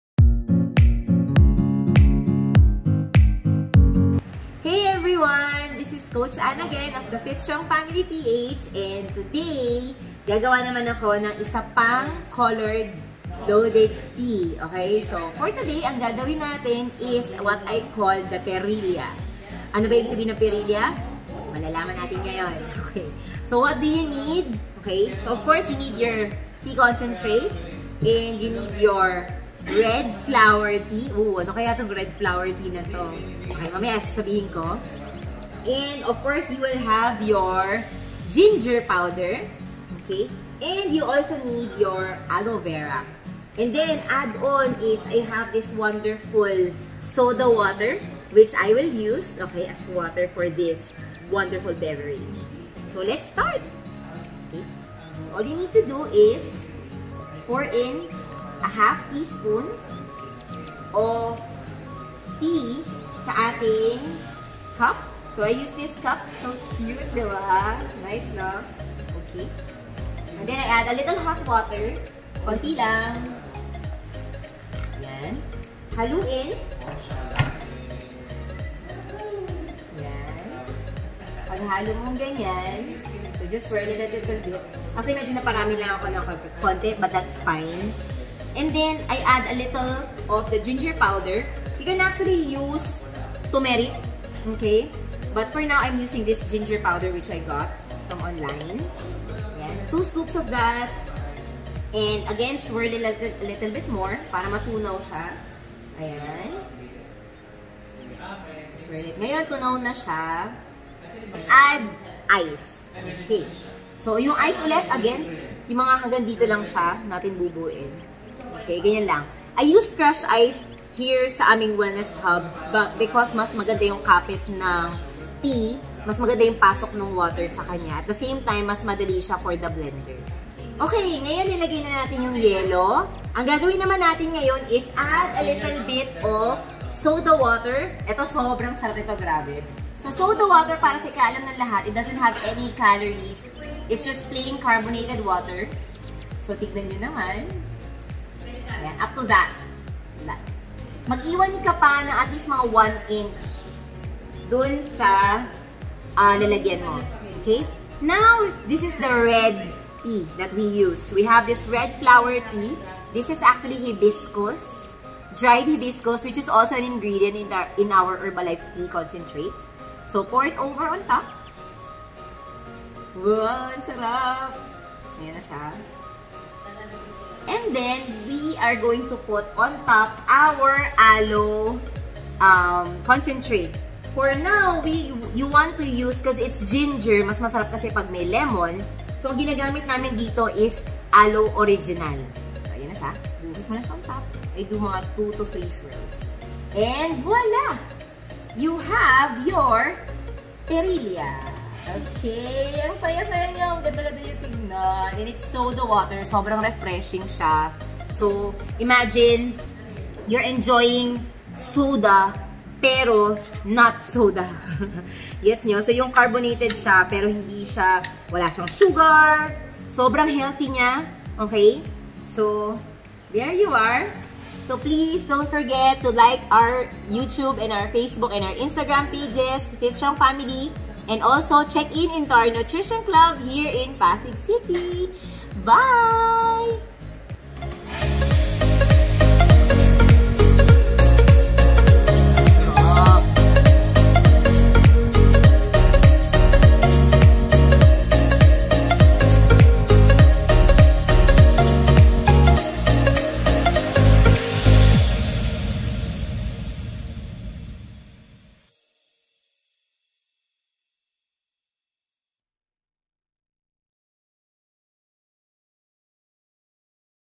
HOW TO MAKE RED PERILLA LOADED TEA WITH HERBALIFE NUTRITION
Red Perilla Loaded Tea
By Coach Anne Aniag

Ingredients:
HERBALIFE Tea Concentrate
HERBALIFE Aloe Concentrate (original)
Turmeric or Ginger Powder
Soda water
Ice
Red Flower Tea (hibiscus)